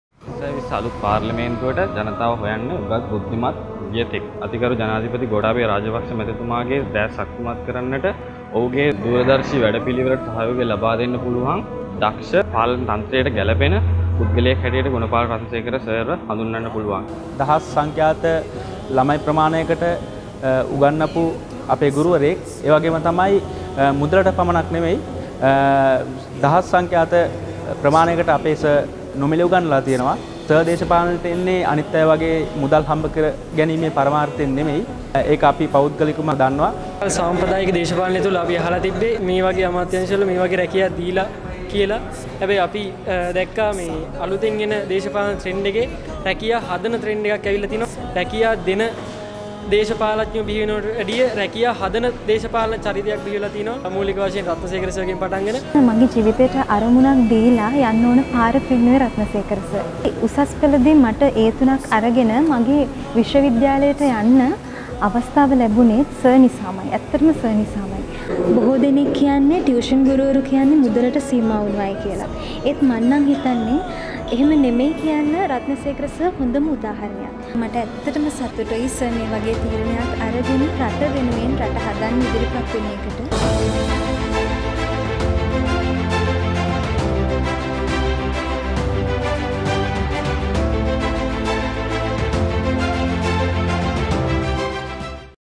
තාරුණ්‍යයේ හදගැස්ම අපේ සර්ටයි..
ගෝඨාභය මහතාගේ දූරදර්ශි වැඩපිළිවෙළට සහය ලබාදෙන්න පුලුවන් , දක්ෂ, පාලන තන්ත්‍රයට ගැලපෙන පුද්ගලයෙක් තමයි රත්නසේකර සර්..